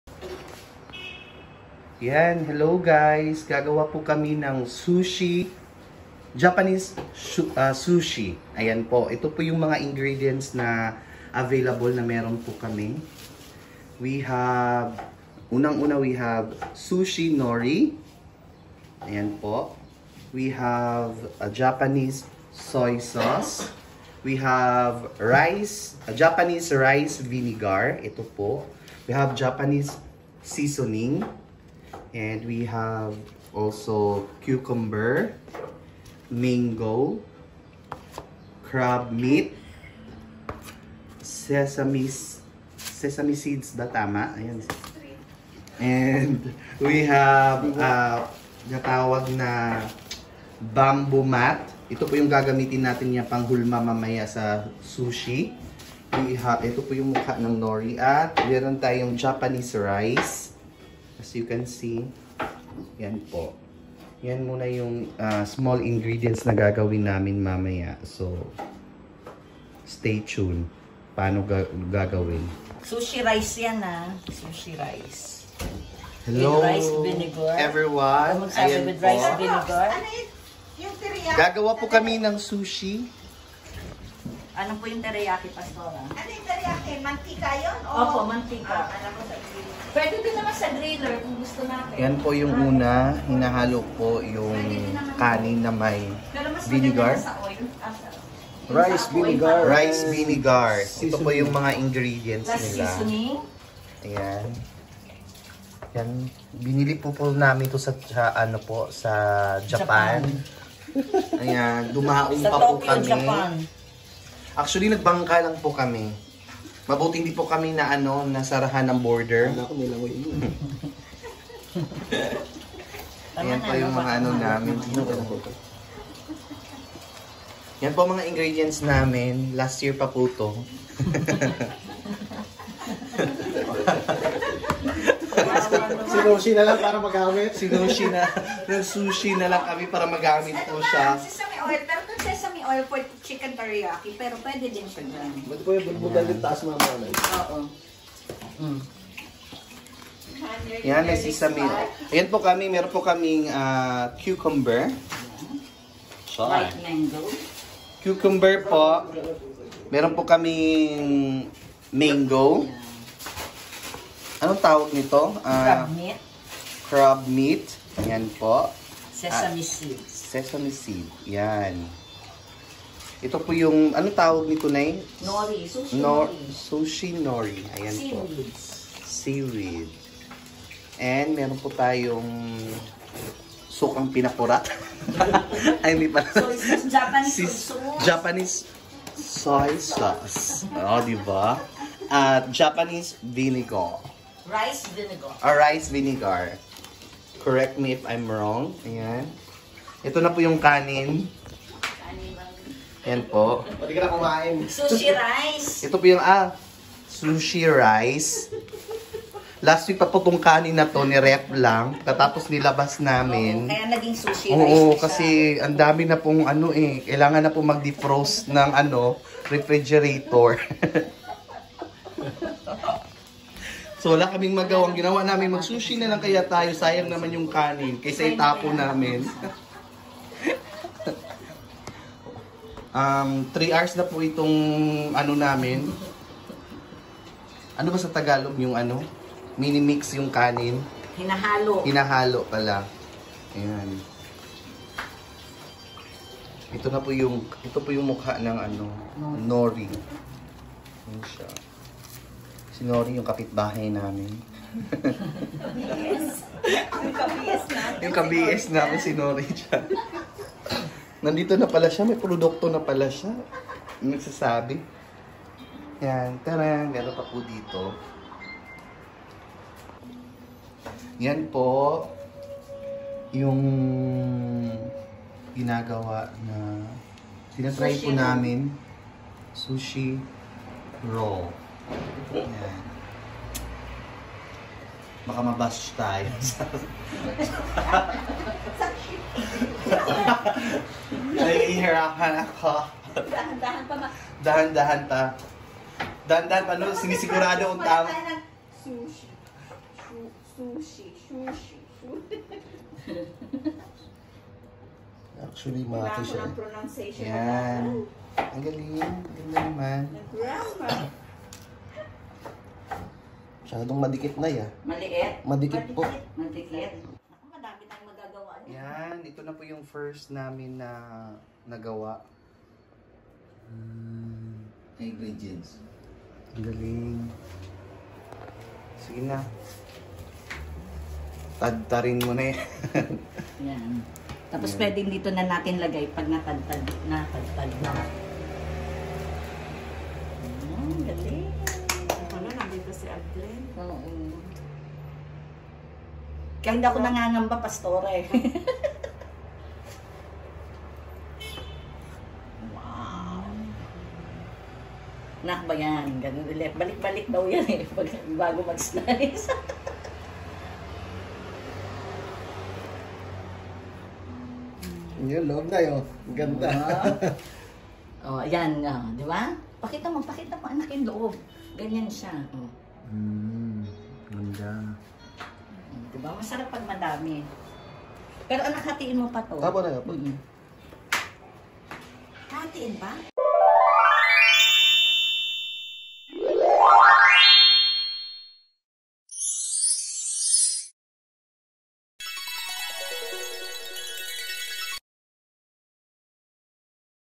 Homemade Sushi 🍱
Try namin gumawa ng homemade sushi kasi mahal sa labas kung bumili 😂 Tipid at sulit pa